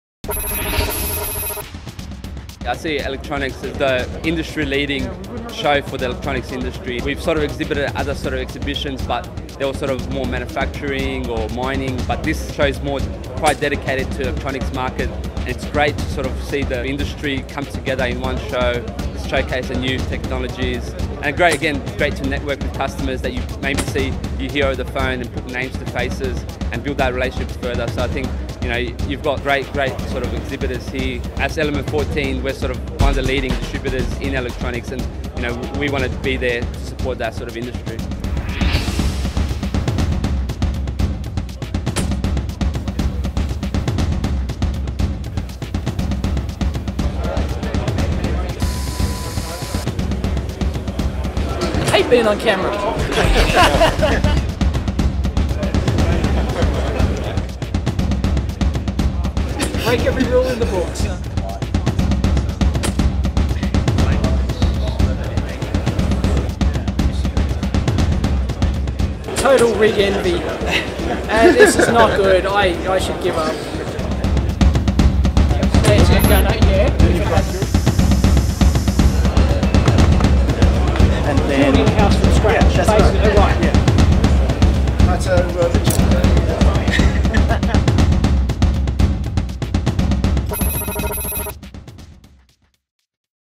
Snapshot of Electronex 2011
A quick look at the "Electronex" trade show held in Melbourne in 2011 with a quick intro by Damir Pletikosa from Element14. I ran into Dave L Jones from the EEVBlog and we duelled cameras for a while! Also caught up with Marc Alexander and Jonathan Oxer from Freetronics. All in all, a great couple of days!